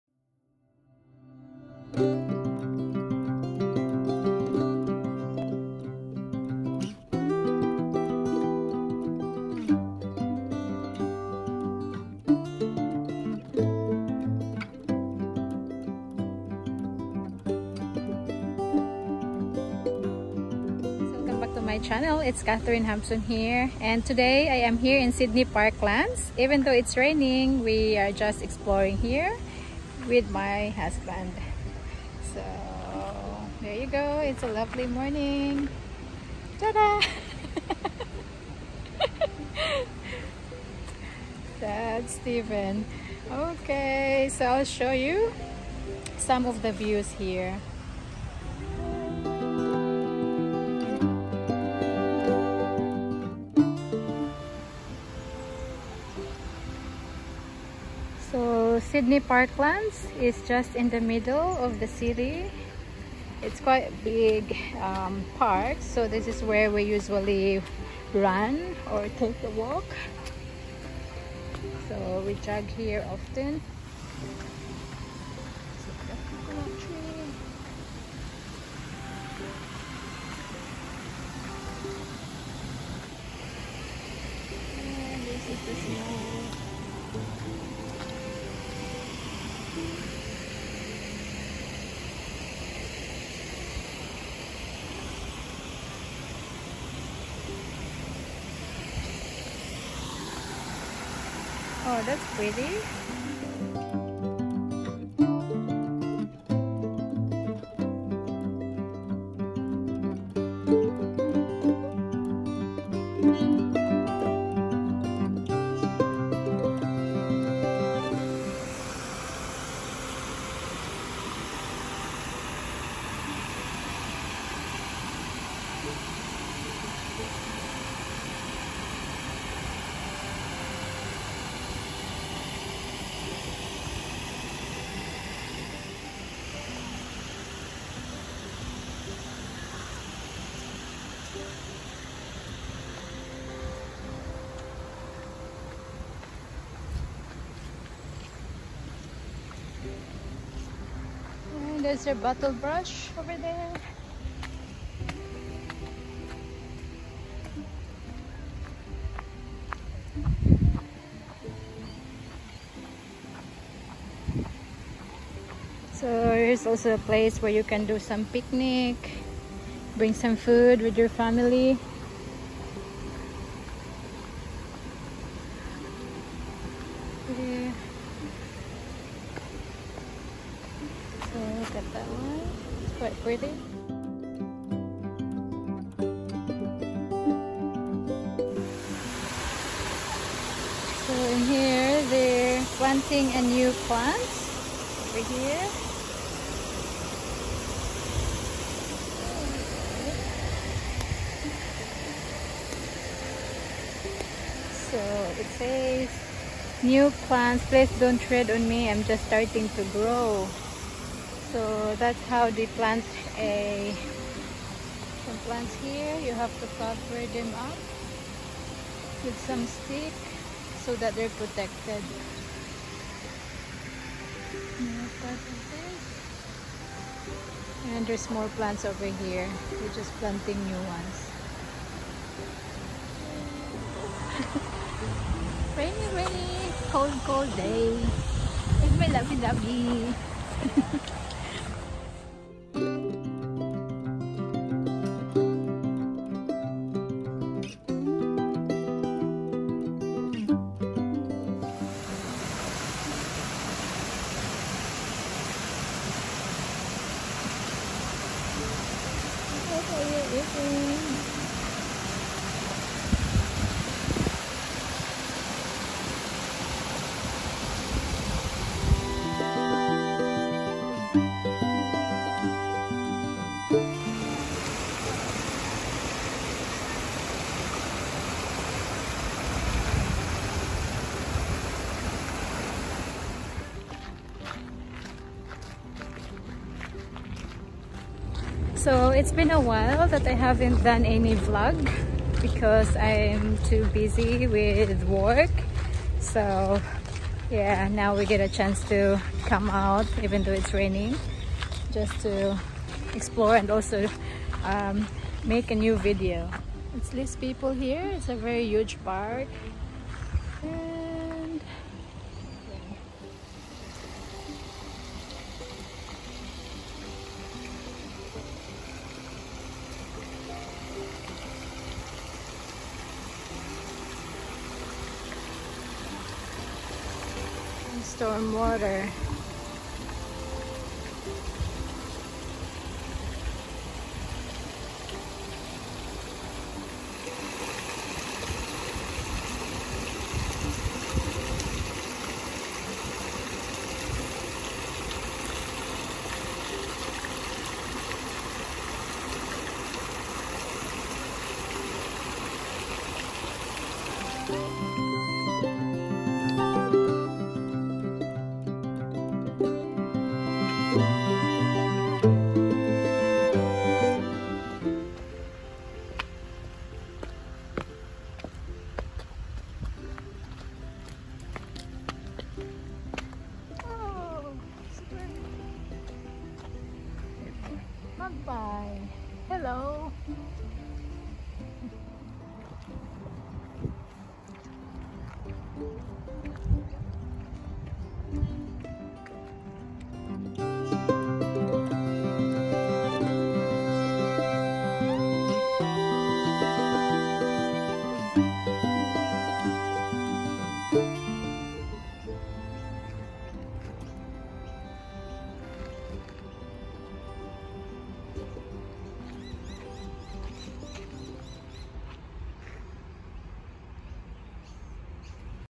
Relaxing, walking and exploring In Sydney Park with My husband | Catherine Hampson
A walk in the park to enjoy the peace and quiet on a wet Saturday